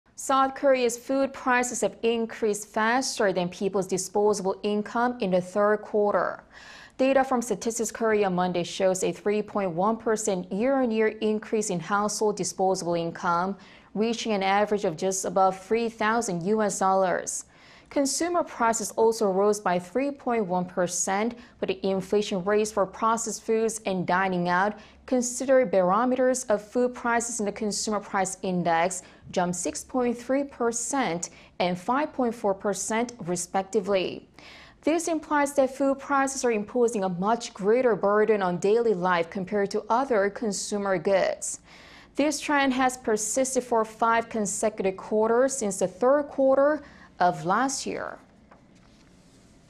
S. Korea's food price increases outpace growth in household disposable income for 5 consecutive ...
S. Korea's food price increases outpace growth in household disposable income for 5 consecutive quarters

3분기 소비•저축여력 3.1% 늘었는데 먹거리물가 5∼6%대 상승

South Korea's food prices have increased faster than people's disposable income in the third quarter.
Data from Statistics Korea on Monday shows a 3-point-1 percent year-on-year increase in household disposable income, reaching an average of just above 3 thousand U.S. dollars.
Consumer prices also rose by 3-point-1 percent, but the inflation rates for processed foods and dining out --considered barometers of food prices in the consumer price index --jumped 6-point-3 percent and 5-point-4 percent, respectively.
This implies that food prices are imposing a much greater burden on daily life compared to other consumer goods.
This trend has persisted for five consecutive quarters since the third quarter of last year.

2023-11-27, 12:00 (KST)